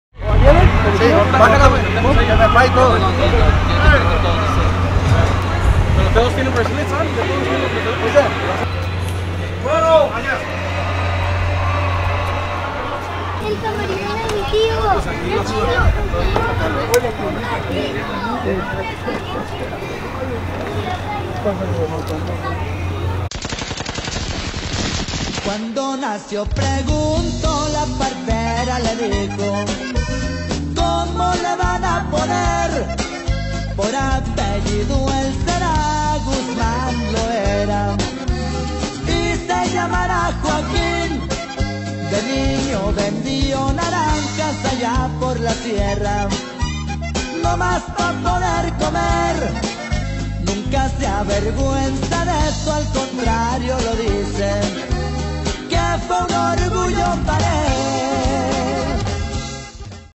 Roberto Tapia at the S.M.O Festival
Behind The Scenes at the Smoke Me Out Festival in Long Beach, CA with Roberto Tapia. Shot By @ErickB.Photography